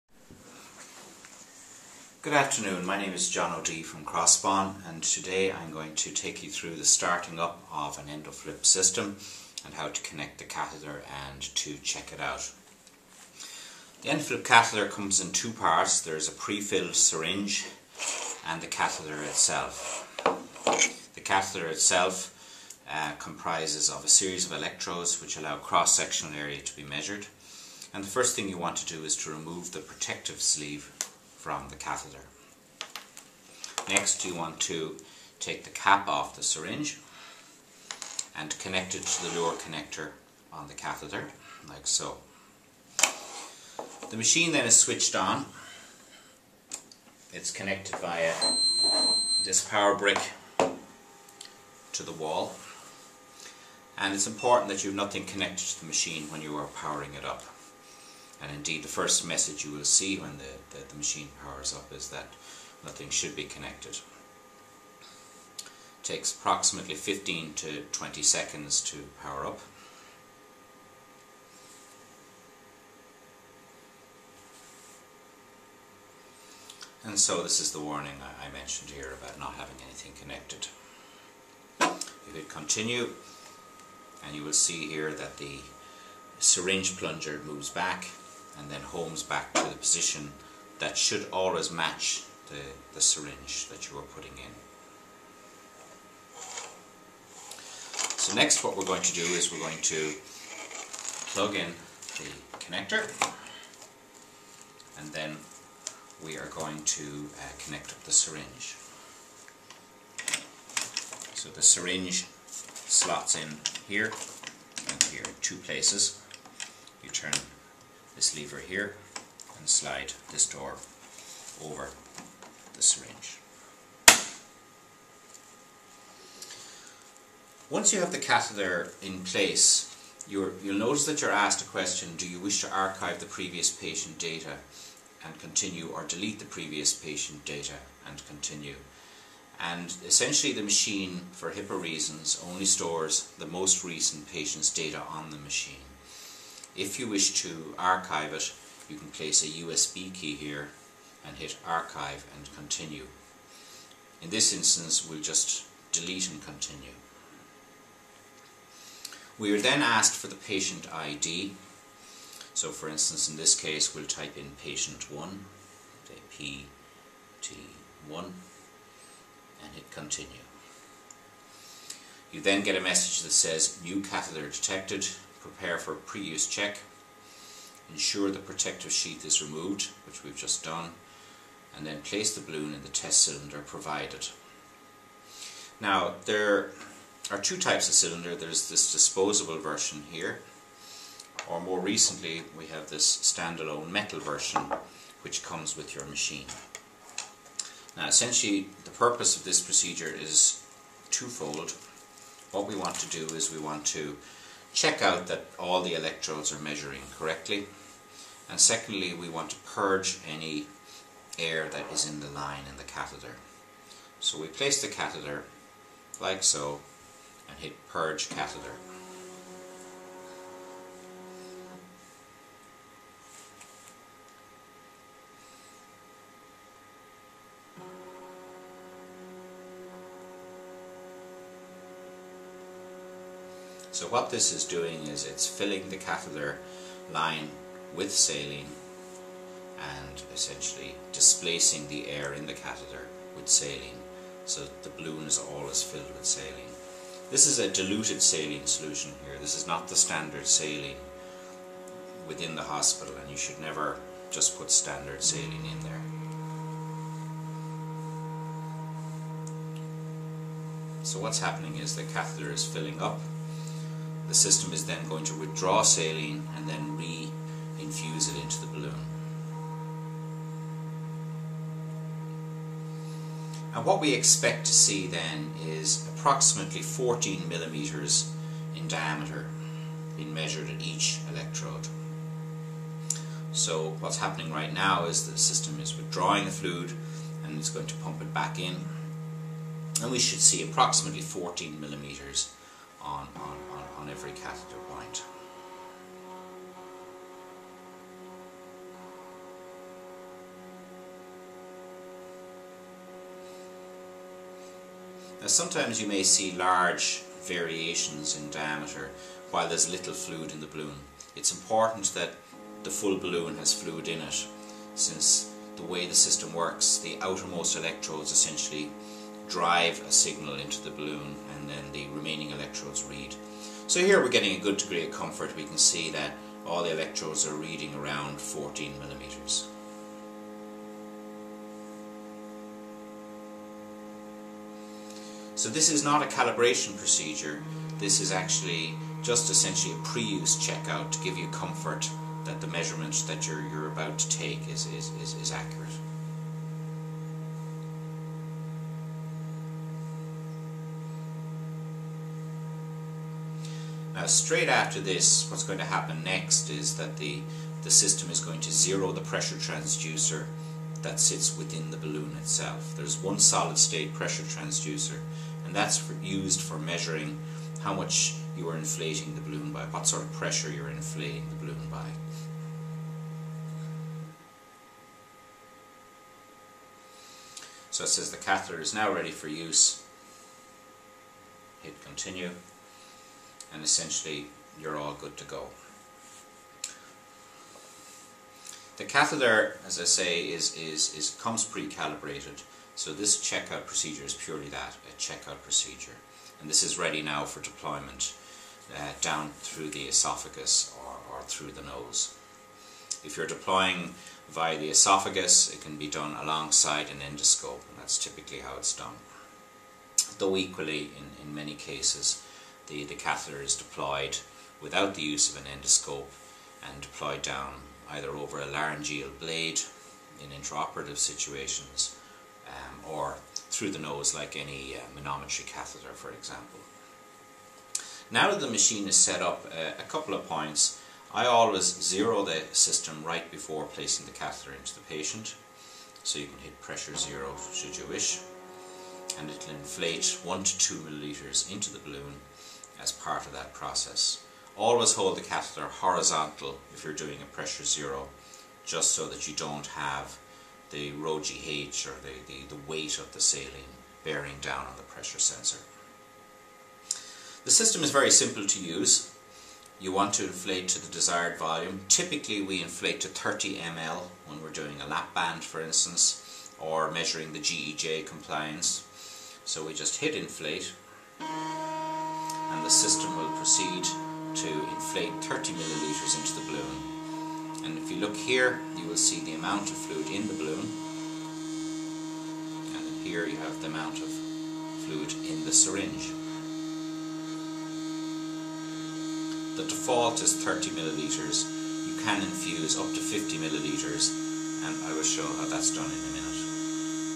Crospon EndoFLIP Setup Part 1 of 2
This is part 1 of a training video on how to set up the Crospon EndoFLIP system